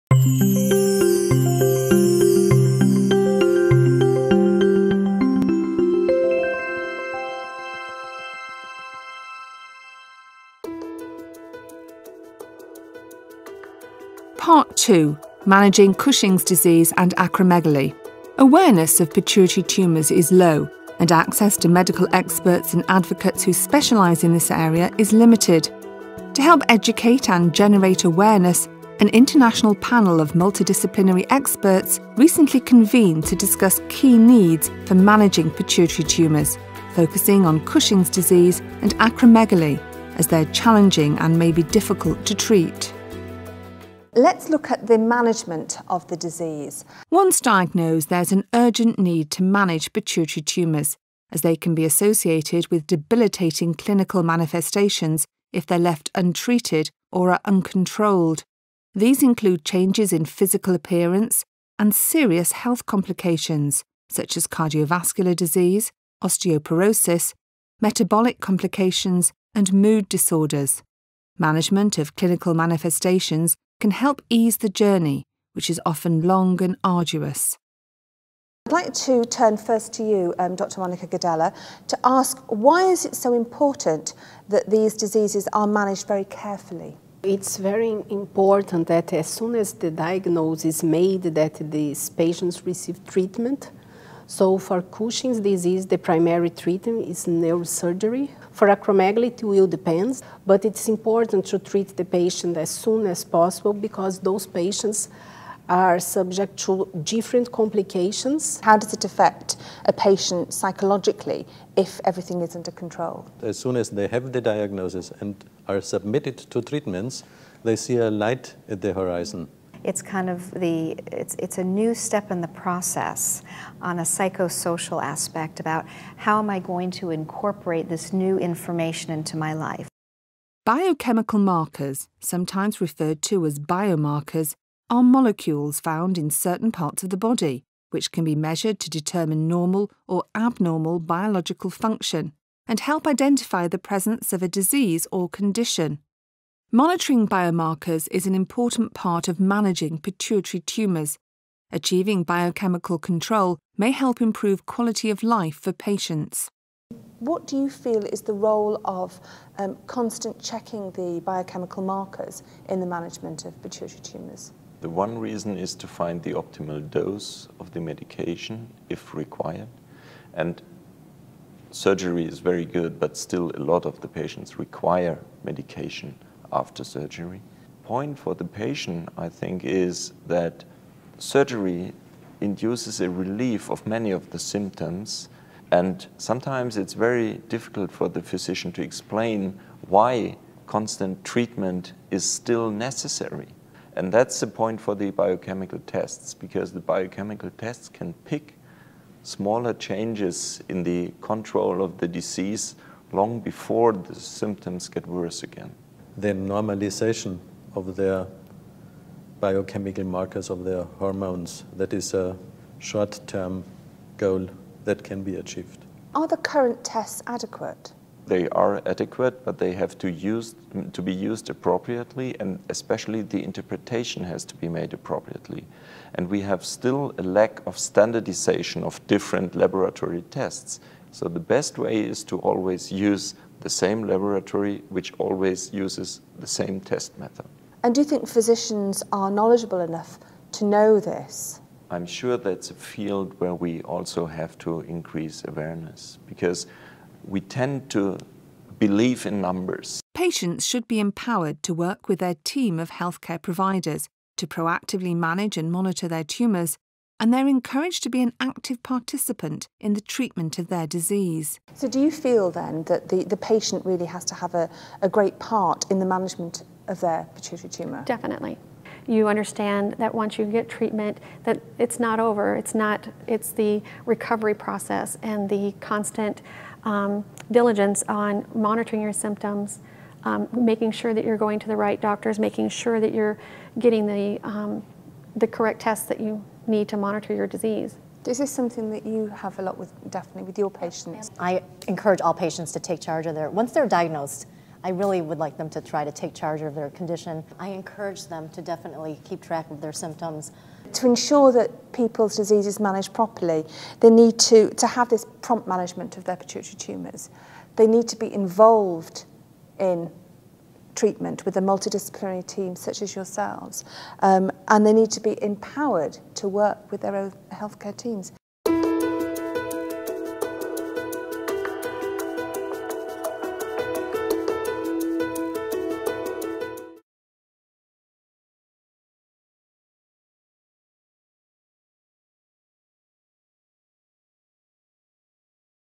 Pituitary Tumor Roundtable - Part Two: Managing Cushing's Disease and Acromegaly
Novartis is committed to supporting the pituitary community and continues to address the evolving needs of patients and caregivers. In this video, a multidisciplinary panel discusses the importance of managing acromegaly and Cushing's disease.